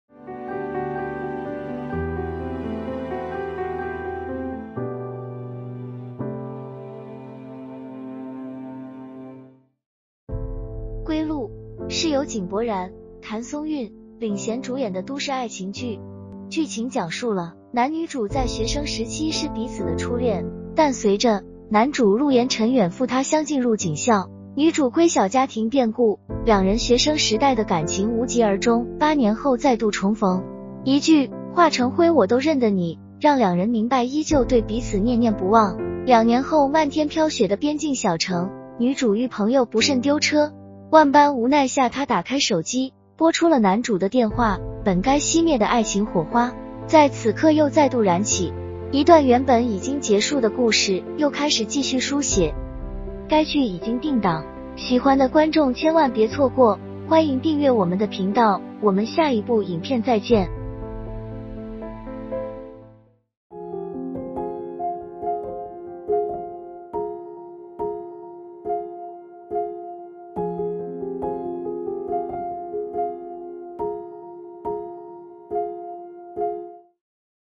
井柏然 譚松韻《歸路》定檔! 初戀重逢的味道!一路美到世界鏡頭~|Tien Channel
井柏然 譚松韻《歸路》定檔! 初戀重逢的味道!一路美到世界鏡頭~
該劇定檔!將在月13日開播